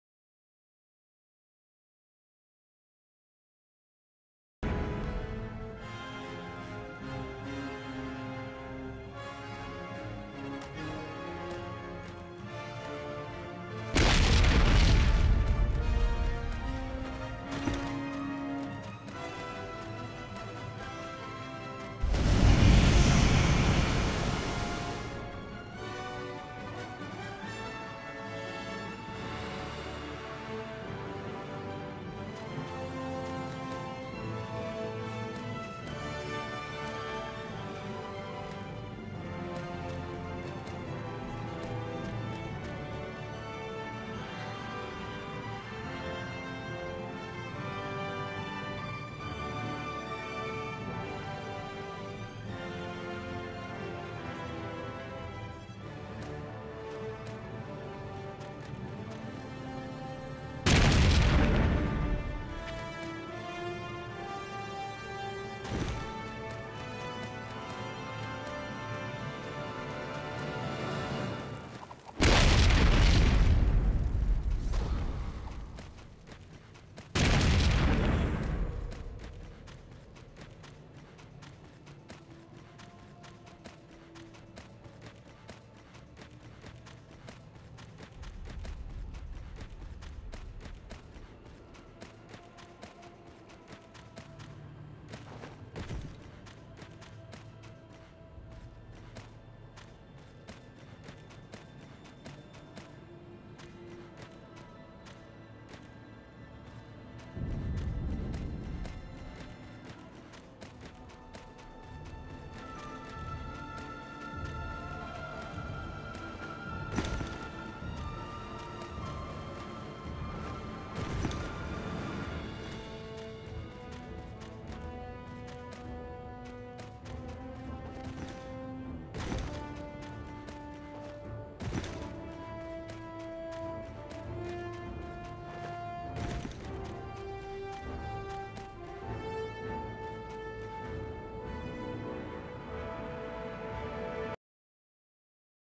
Out Of Map - Minas Morgul Htdm (Tunngle Gameplay)
Glitched out of map in Htdm Minas Morgul